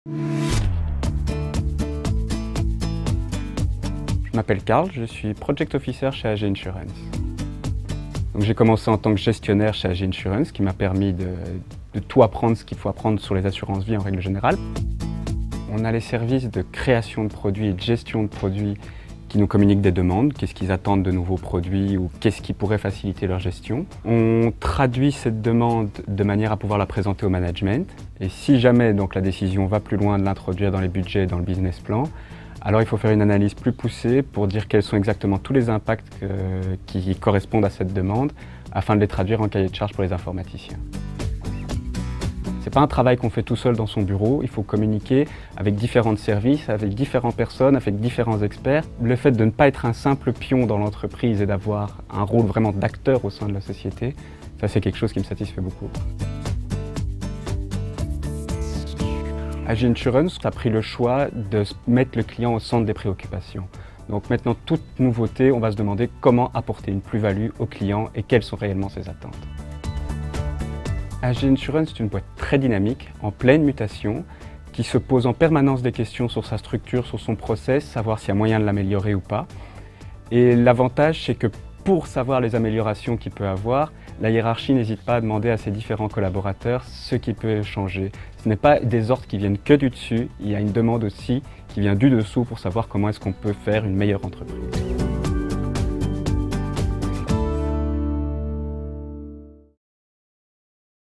AG Insurance: Discover Karl, Project Officer
Karl est Project Officer dans le département qui développe de nouveaux produits d'assurance vie. Il donne des présentations de produits à la direction et rédige des cahiers des charges pour ses collègues d'IT. Envie de savoir ce qu'il pense de la culture d'entreprise d'AG Insurance ? Regardez ici sa vidéo ! Et découvrez ensuite directement comment devenir collègue de Karl sur jobs.aginsurance.be

Karl is Project Officer in de afdeling die nieuwe producten ontwikkelt binnen levensverzekeringen. Hij presenteert productvoorstellen aan de directie en stelt een lastenboek op voor de collega's van IT. Benieuwd hoe hij de bedrijfscultuur ervaart bij AG Insurance? Bekijk dan hier zijn video! En ontdek daarna meteen hoe je Karl's collega wordt op jobs.aginsurance.be